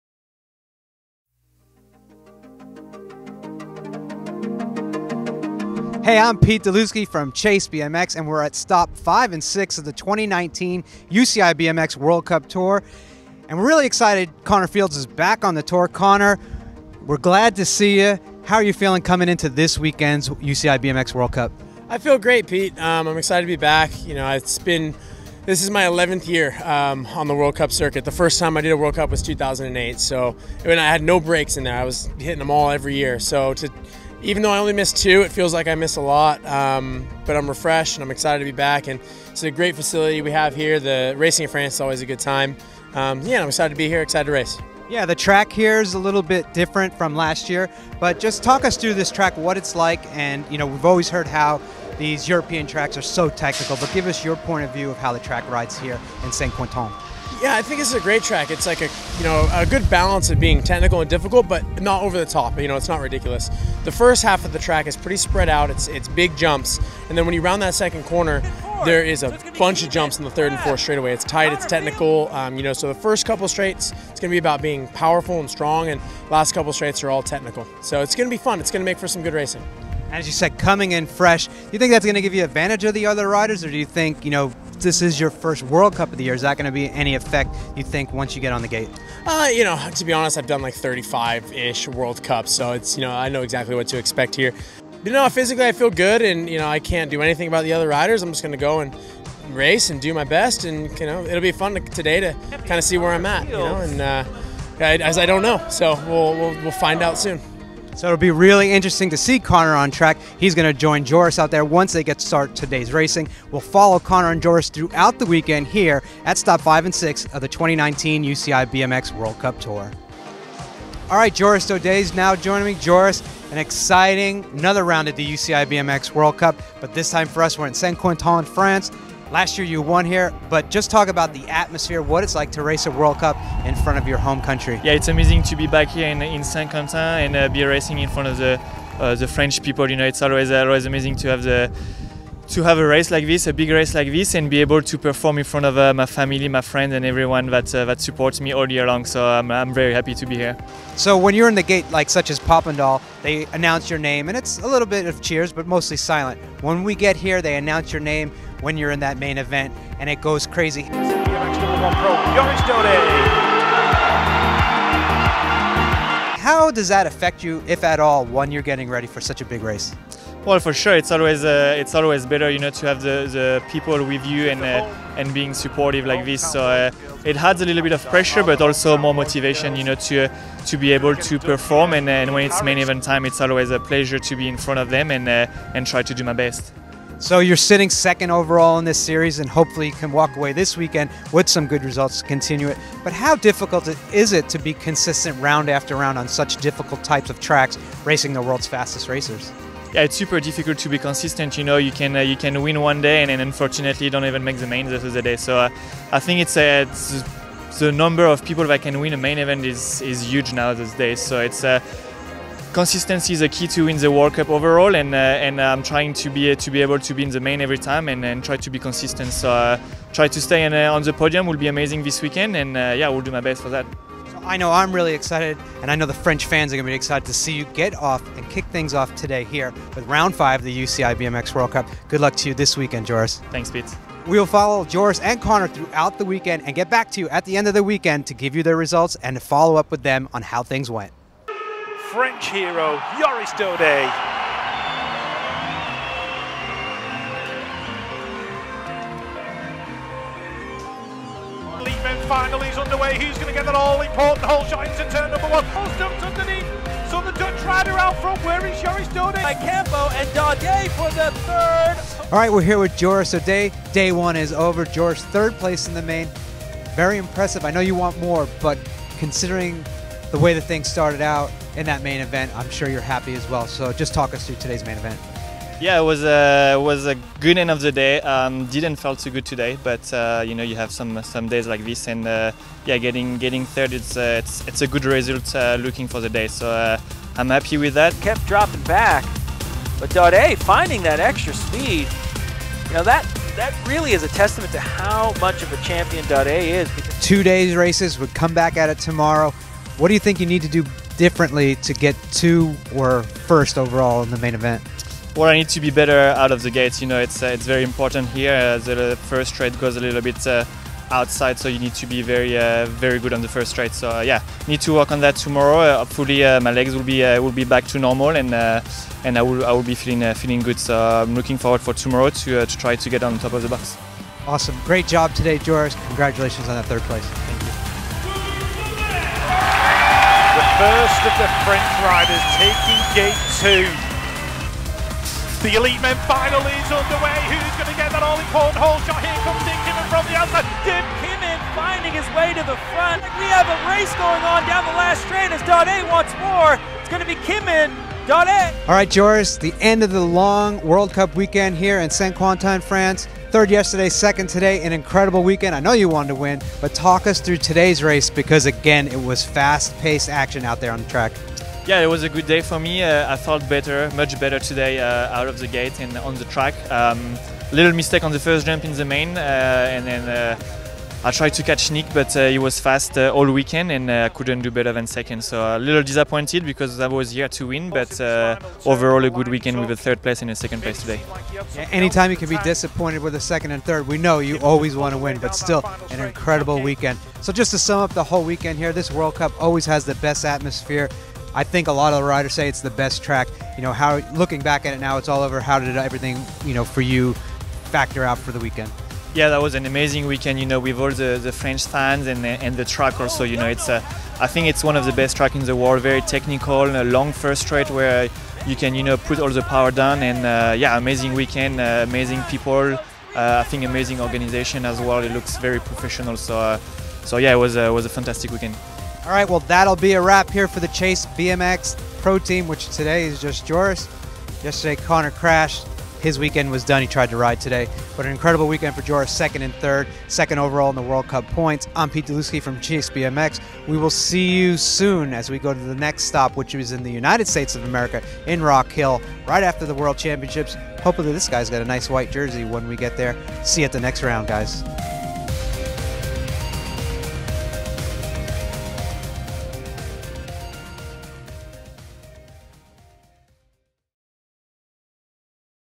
Chase BMX UCI BMX Supercross World Cup Recap - SQY
From the BMXRacingGroup Youtube page -

Chase BMX Pro team Race Report - Rounds 5 & 6 of the 2019 UCI BMX World Cup Supercross tour were held in Pairs, France and both Joris Daudet and Connor Fields were ready to race against the world fastest racers. Follow the journey of both racers through the race weekend and see how they wound up.